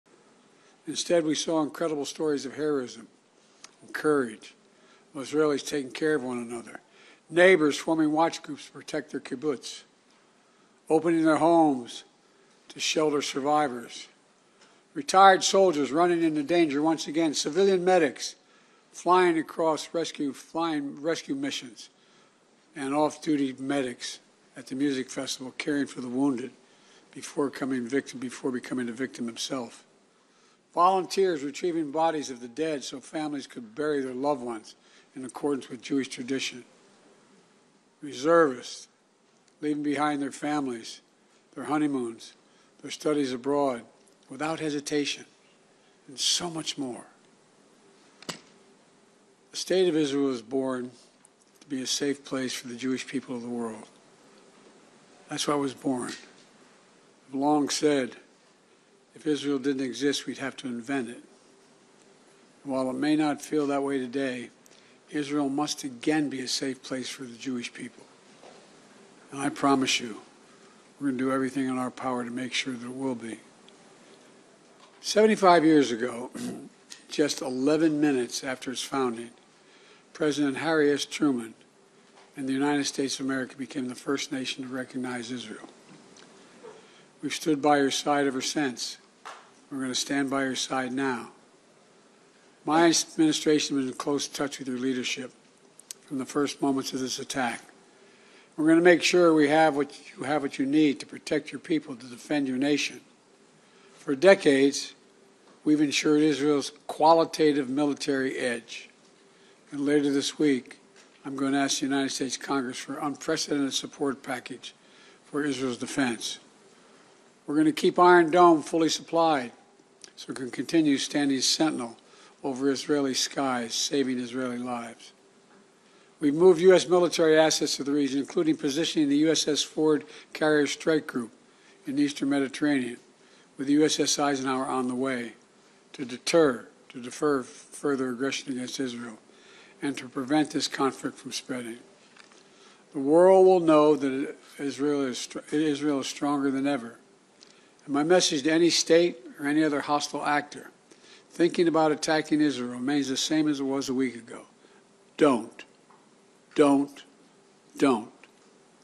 Watch: Biden Pledges To Ask ‘Congress For An Unprecedented Support Package For Israel’s Defense’
On Wednesday, President Biden discussed Congressional allocation of funding to Israel during remarks delivered in Tel Aviv.